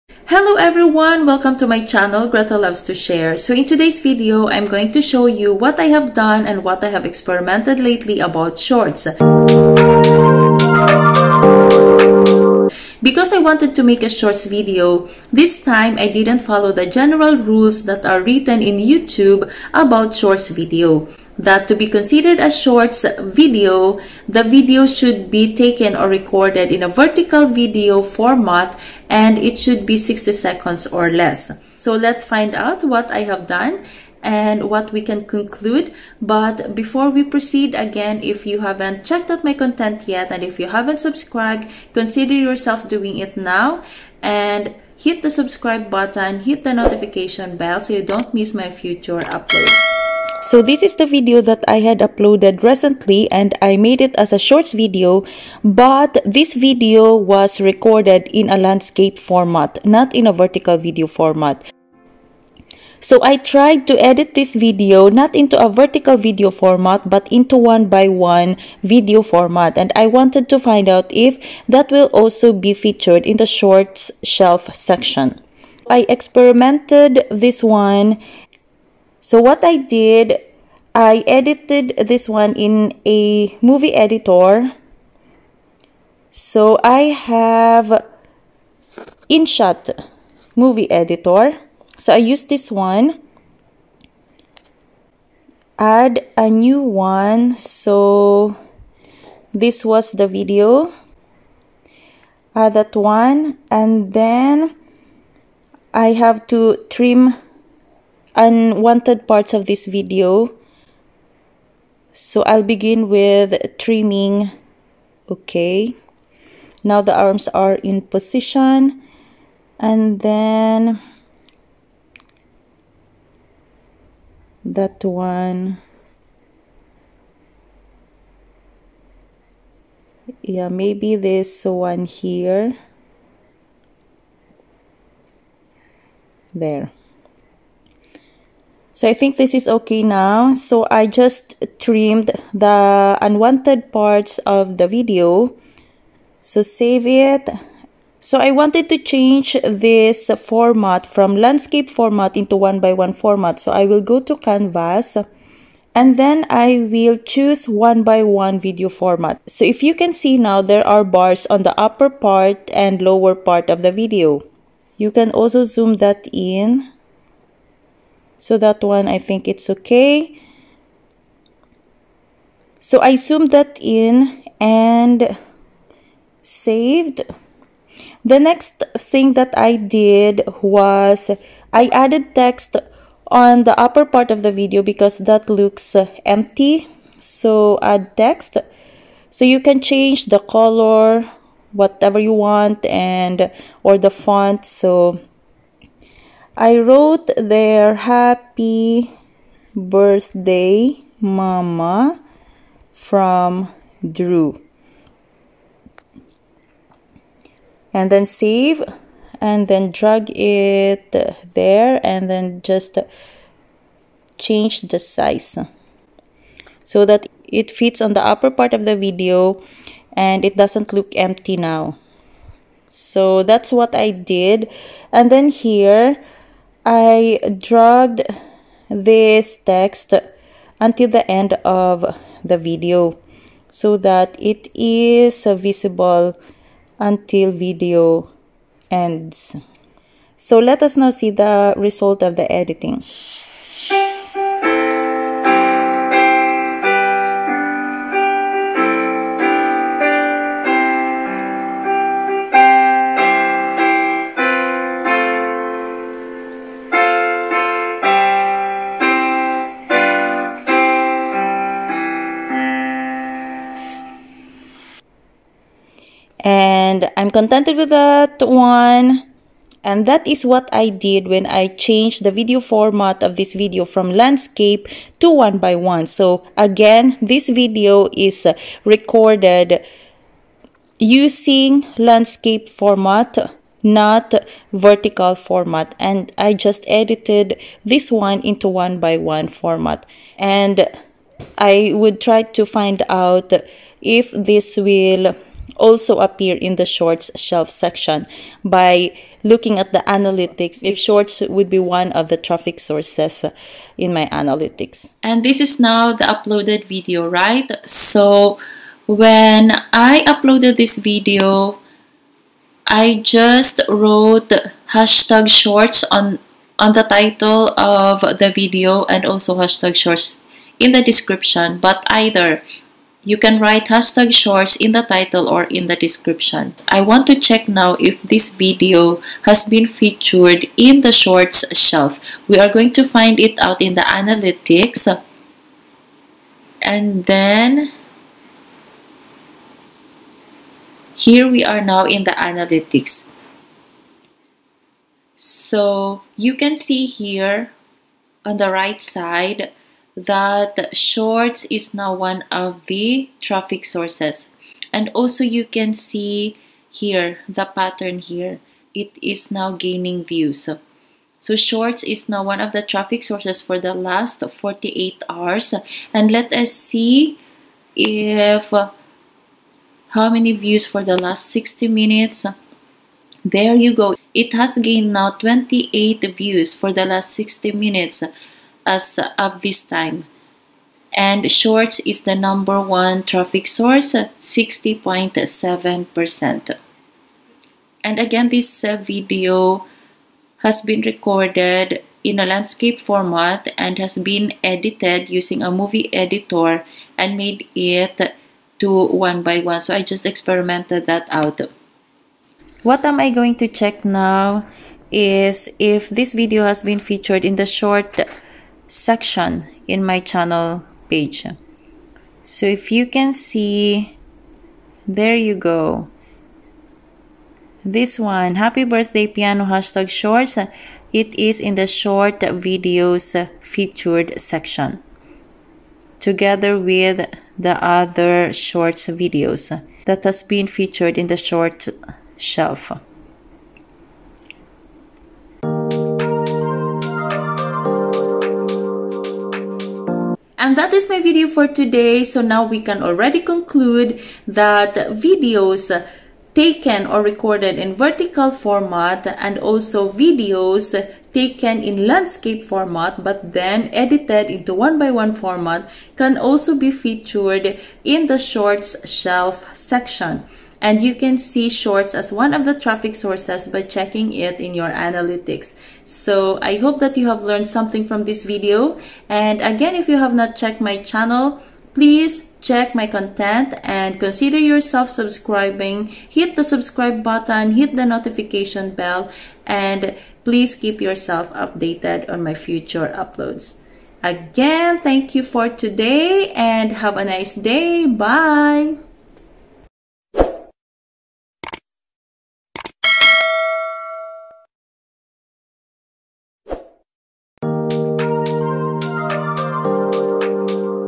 How To Edit Video For Youtube Shorts
How To Edit Video For Youtube Shorts? I experimented a video recorded in a landscape format, edited it and made it as shorts video. How did I make it? Find out by watching the whole video.